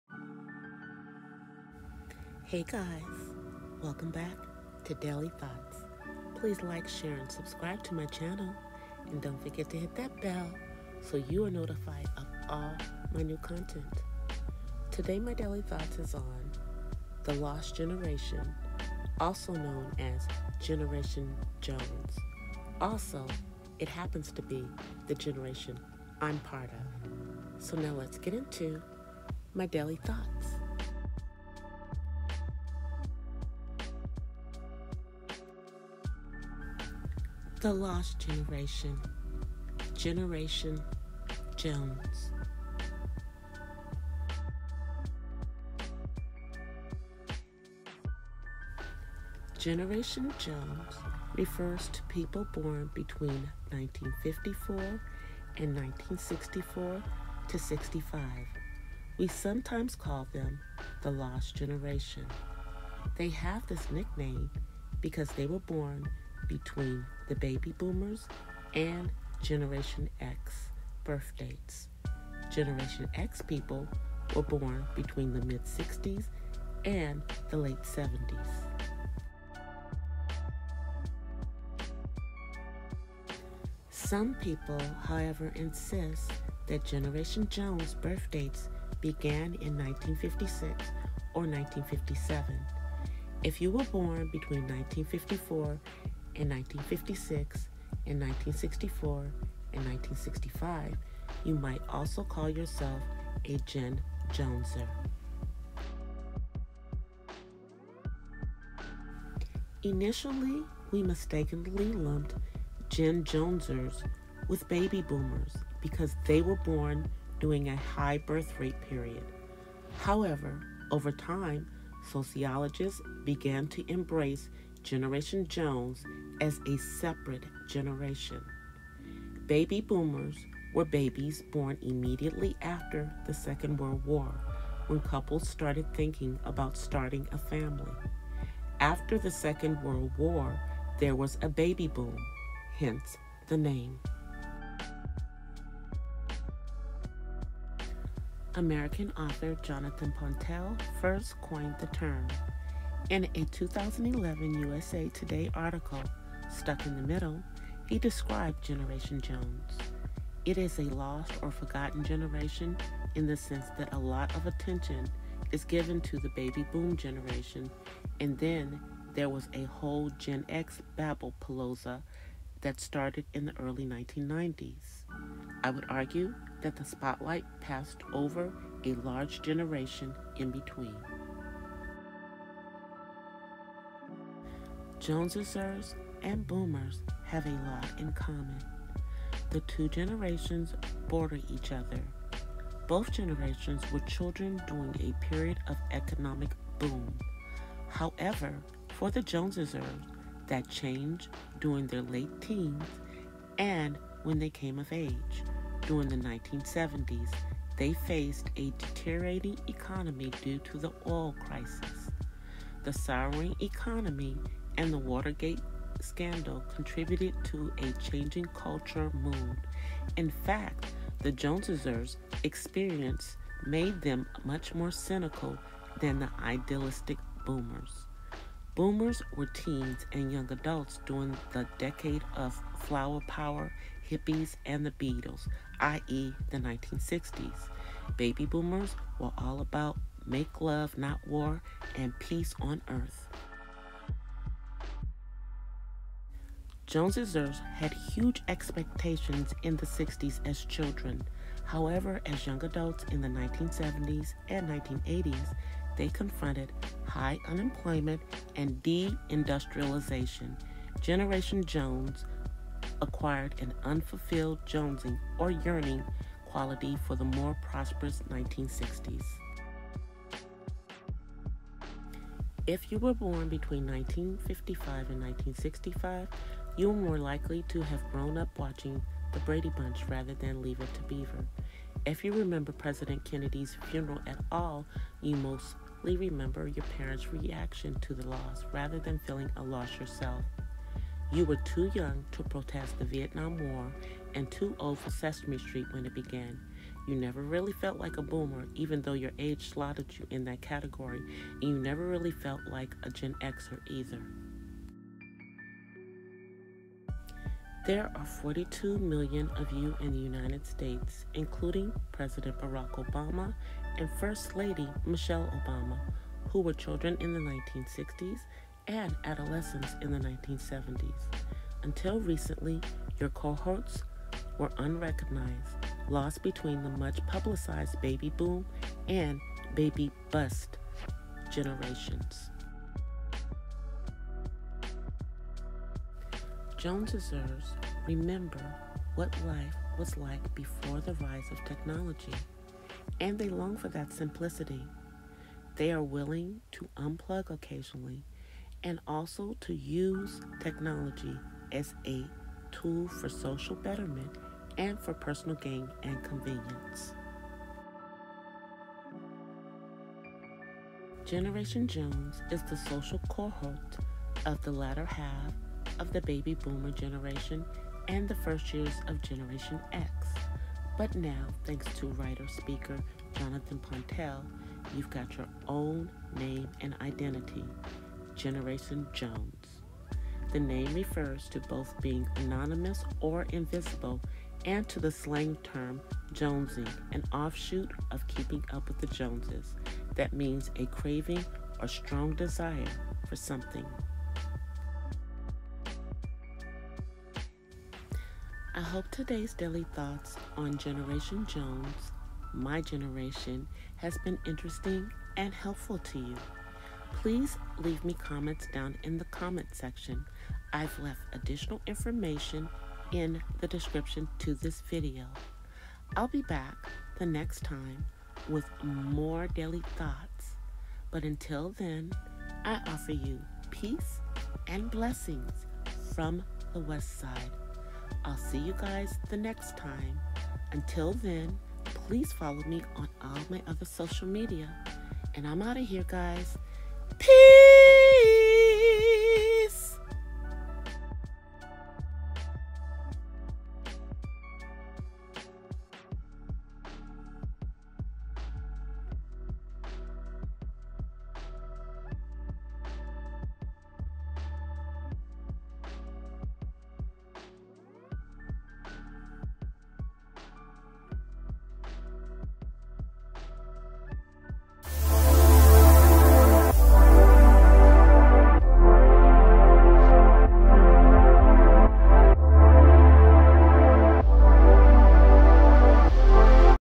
Daily Thoughts ~ The Lost Generation “Generation Jones “
Commentary on The Lost Generation ~ Generation Jones born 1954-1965.

Generation Jones refers to people born between 1954 and 1964-65. We sometimes call them the 'Lost Generation. ' They have this nickname because they were born between the Baby Boomers and Generation X birth dates. Generation X people were born between the mid-1960s and the late 1970s.

Instagram & Snapchat @DCThePurpleDiva
Twitter & Periscope @ThePurpleDivaDC
Facebook: DCThePurpleDiva's Chronicles of an Artsy Autismmom